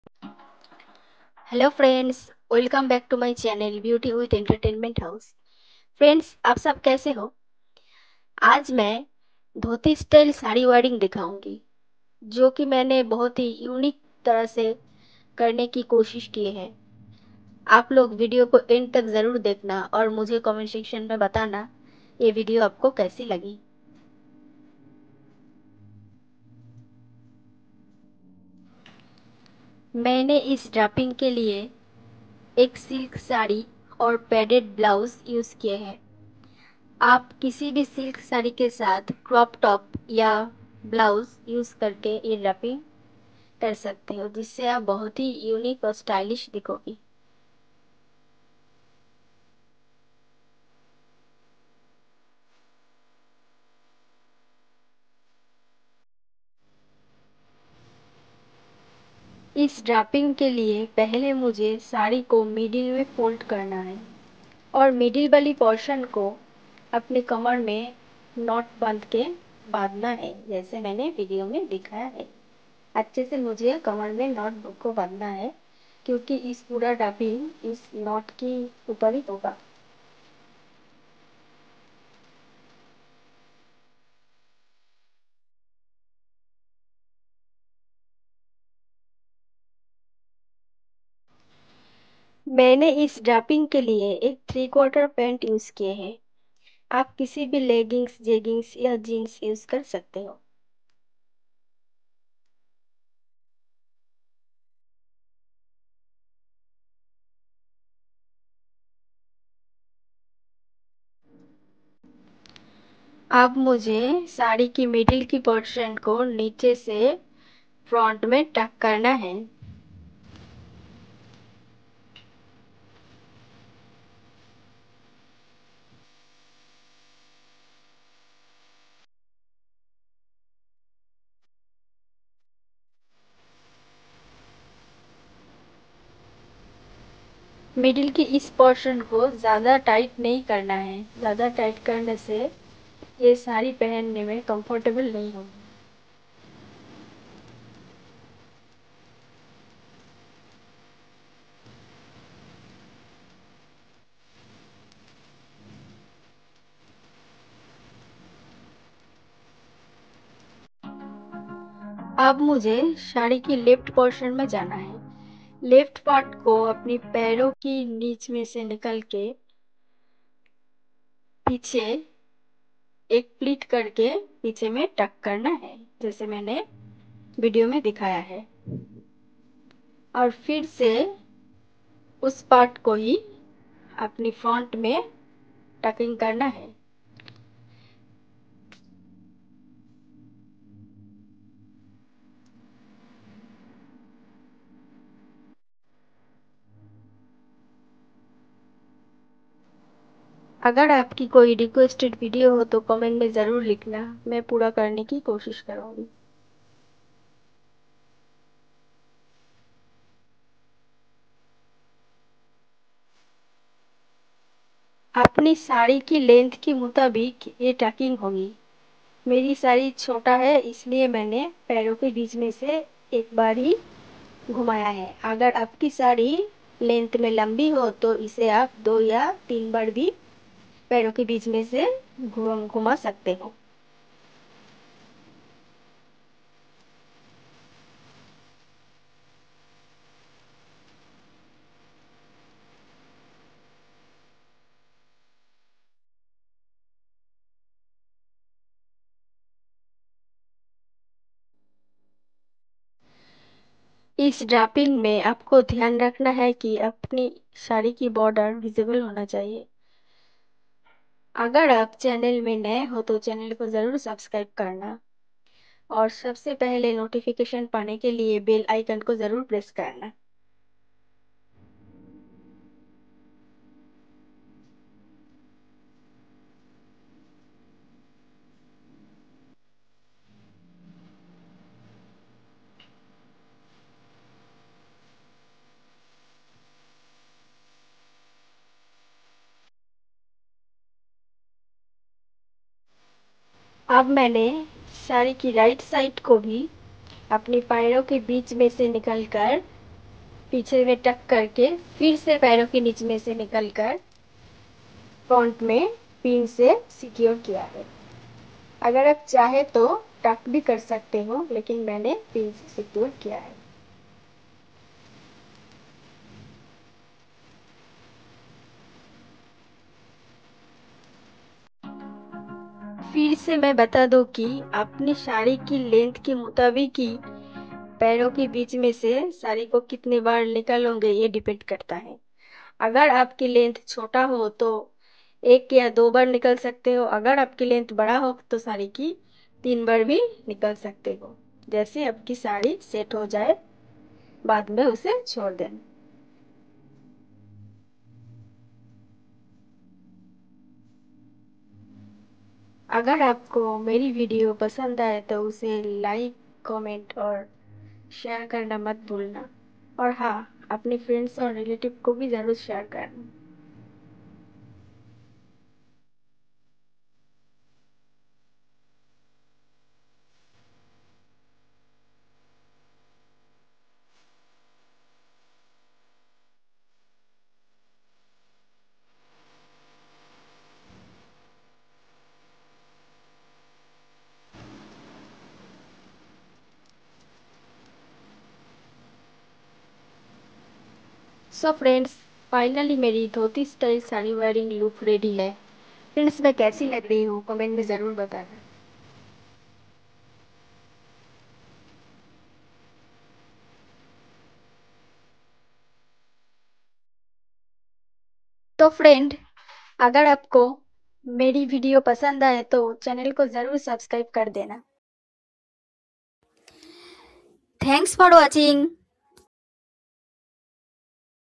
Dhoti Style Saree Draping | Easy Silk Saree Draping Step by Step Tutorial
How to wear perfect Dhoti Style partywearsaree dhotistyle howtowearsaree indiwesternsaree newstylesareedrape wearsaree silk saree draping styles.